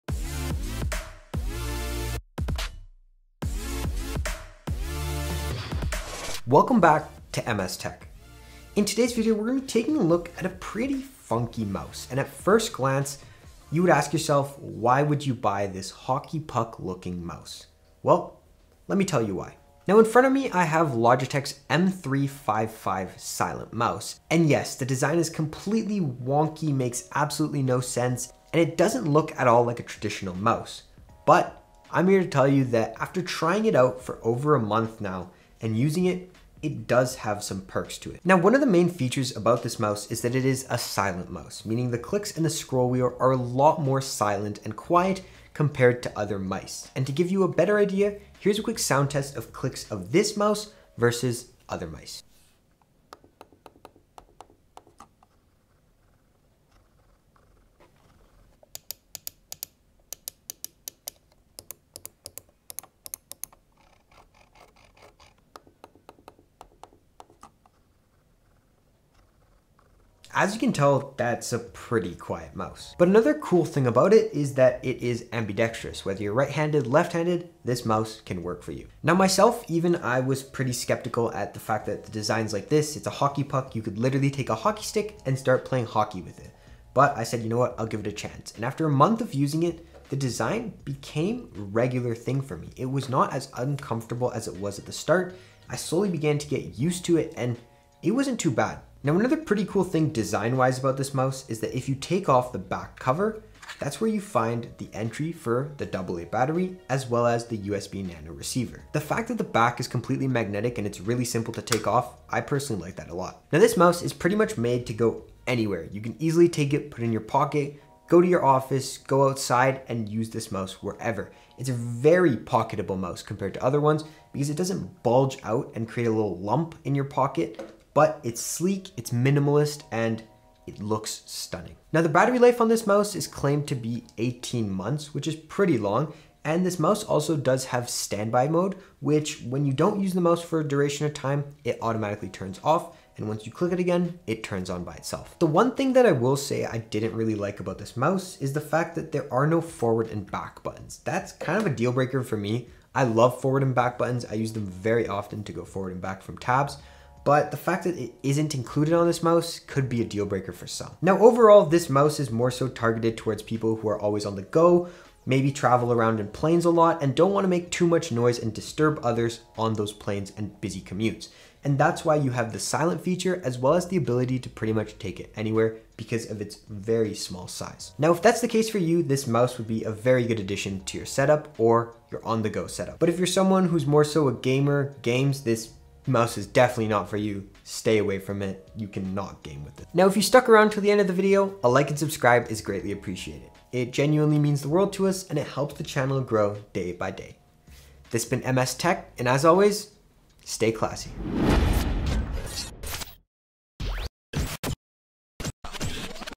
A Strange Looking, Totally Silent, Wireless Mouse! (Logitech M355 Review)
Let's see how the Logitech M355 stacks up in 2021!